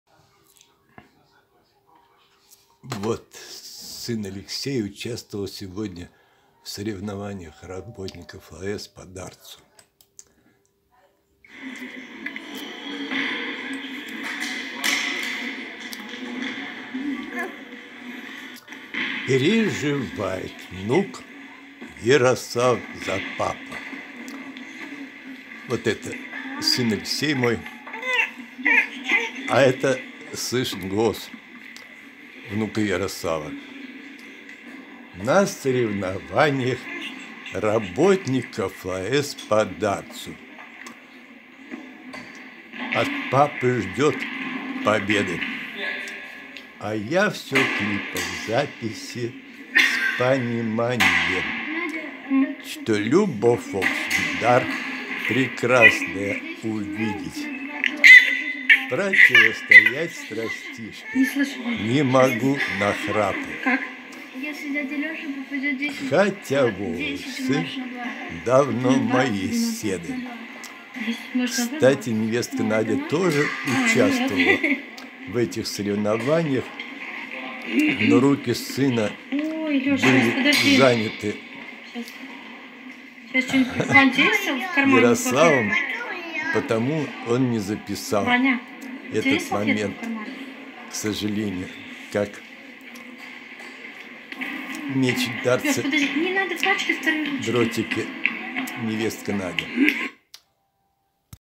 Переживает внук за папу
Переживает внук Ярослав за папу.
На соревнованиях работников ЛАЭС по дартсу от папы ждет победы.
А я все клипов записи с пониманием, что любовь, в общем, – дар прекрасное увидеть, противостоять страстишки не могу нахрапу.
Хотя волосы давно мои седы.